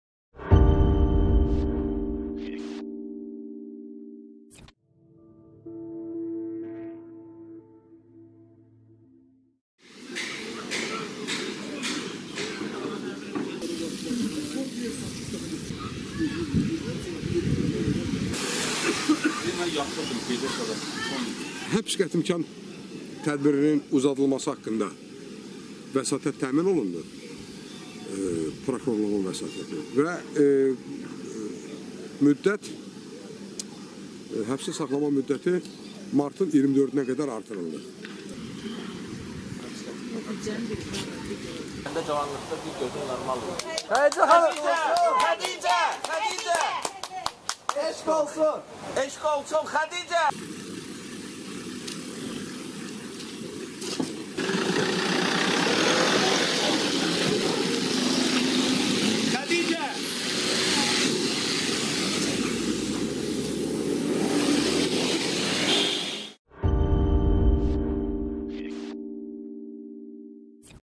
Xədicə İsmayılın həbs müddəti artırıldı
Ətraflı OXU:
Yanvarın 27-də Səbayıl rayon Məhkəməsində həbsdə olan jurnalist Xədicə İsmayılın həbs müddətinin artırılması ilə bağlı təqdimata baxılıb. Prokuror Xədicə İsmayılın həbs müddətinin daha 4 ay uzadılması ilə bağlı təqdimat verib. Müşavirədən qayıdan məhkəmə jurnalistin istintaq müddətində daha 2 ay həbsda saxlanması ilə bağlı qərar verib.

Xədicə İsmayıl ötən il dekabrın 5-də həbs olunub. O, jurnalist Tural Mustafayevin «özünü öldürmə həddinə çatdırma» ittihamı ilə saxlanıb. Xədicə İsmayıl bu ittihamları rədd edərək, həbsini siyasi sifariş sayır.

Azadlıq Radiosunun rəhbərliyi bəyan edib ki, jurnalistin həbsi onun ölkə məmurlarının gizli biznesi haqda araşdırmalarına görə hakimiyyətin qisasıdır.

Fevralın 5-də Xədicə ismayılın həbs müddəti başa çatmalı idi.

Audio: